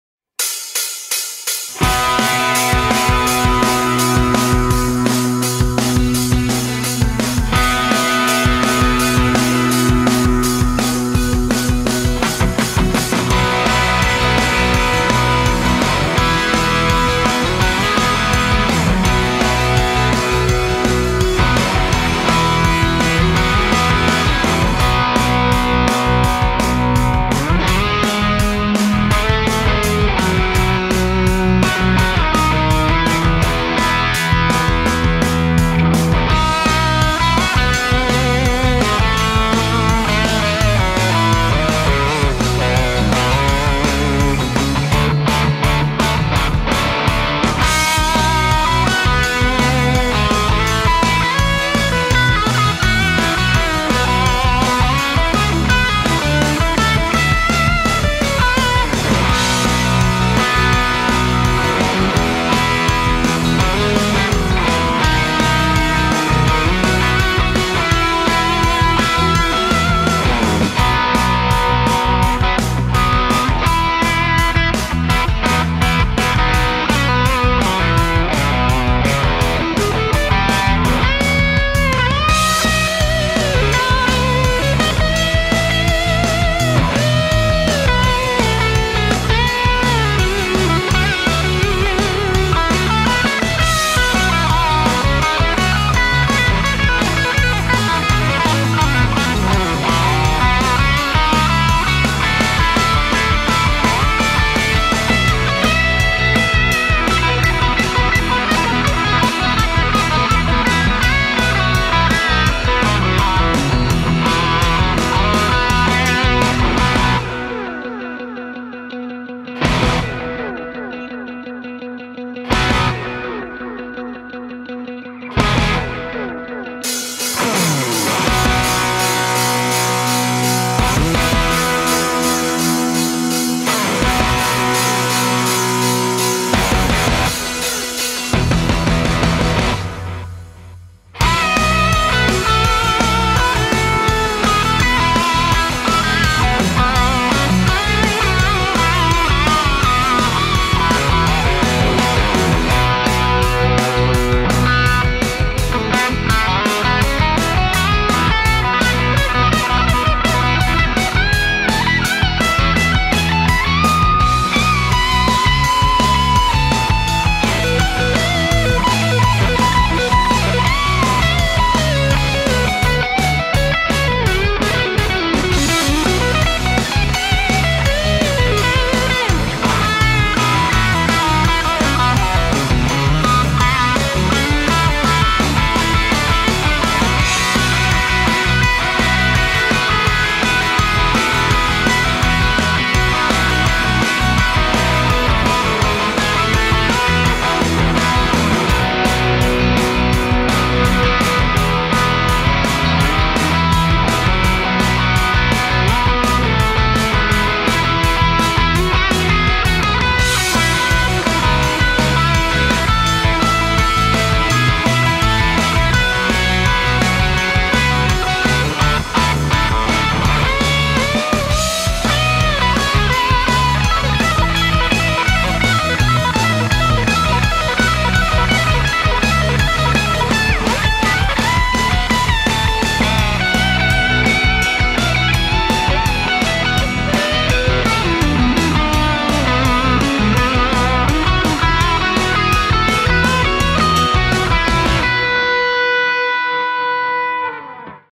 KLD Firewall 12HR and Spear Guitar demo
Demo made for aqua8.nl Played on a KLD Firewall 12HR amp and Spear guitar. Also a PAN amplification AT-100 attenuator is used.